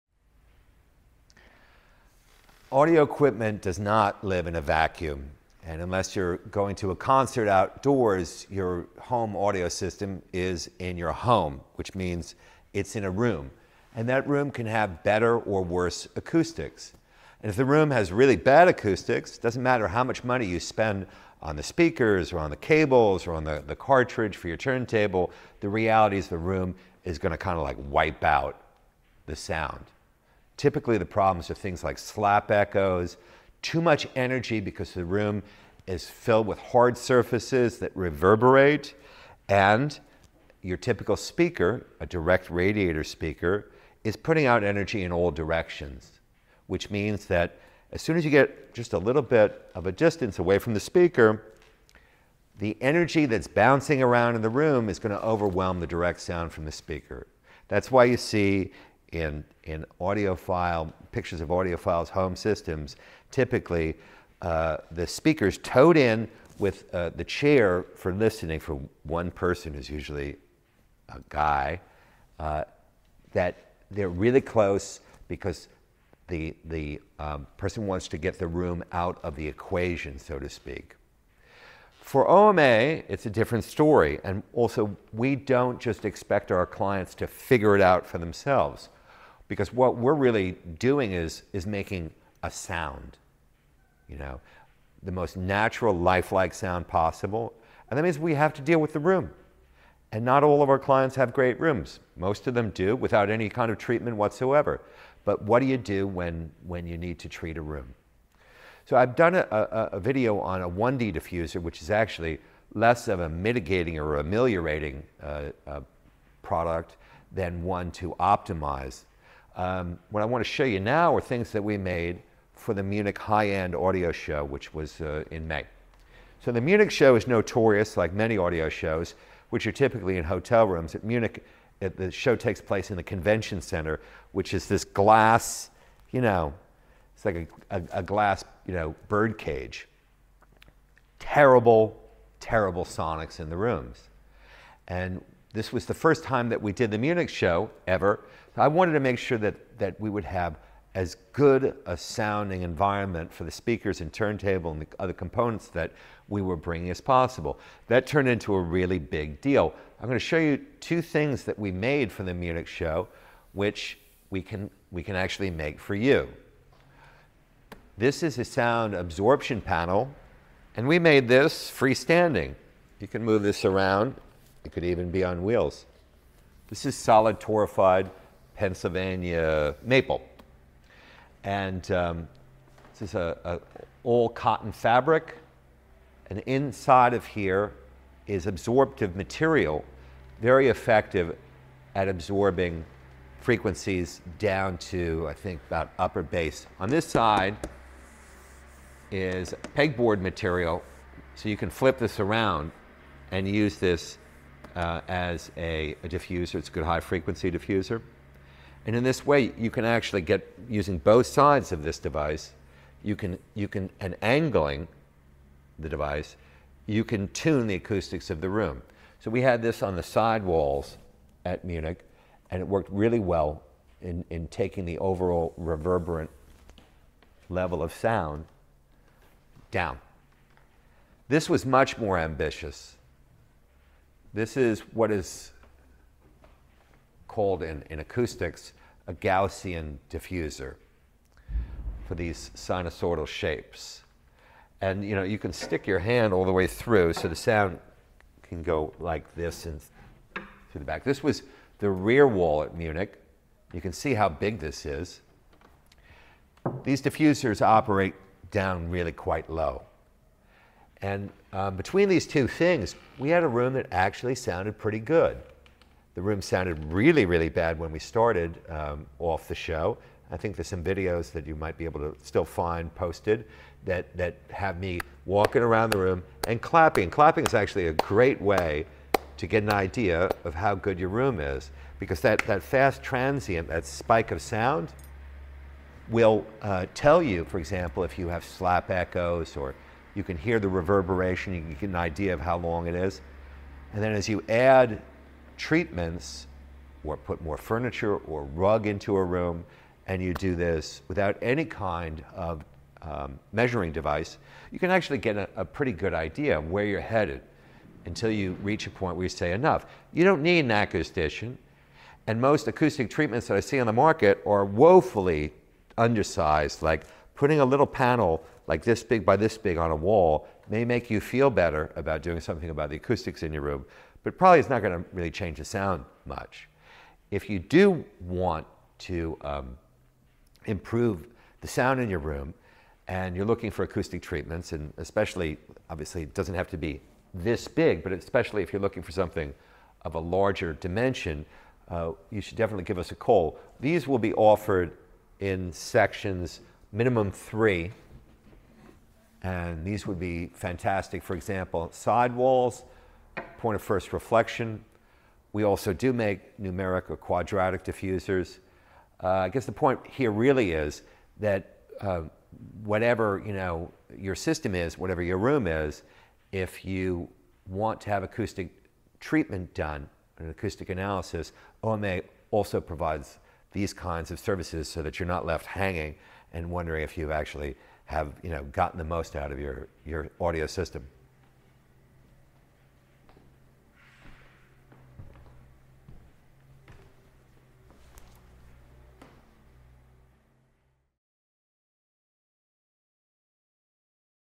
OMA Acoustic Treatments
Not all rooms sound great; we can help you create exactly the kind of listening space you want, no matter what size. Our team of world class acousticians and audio engineers have worked on just about everything: custom installations, home theaters, listening bars and lounges, private clubs and even large concert halls and recording studios. We even design and build several lines of acoustic treatments that can be tailored to your room, interior decor and listening preferences.

Gaussian Diffusor
This diffusor type can be used on room boundaries or free standing, and operates lower in frequency than the 1D Diffusor.

Absorption Panels
These panels have two sides, with differing levels of absorption, to provide maximum flexibility. Available free standing or wall mounted. Specs on request.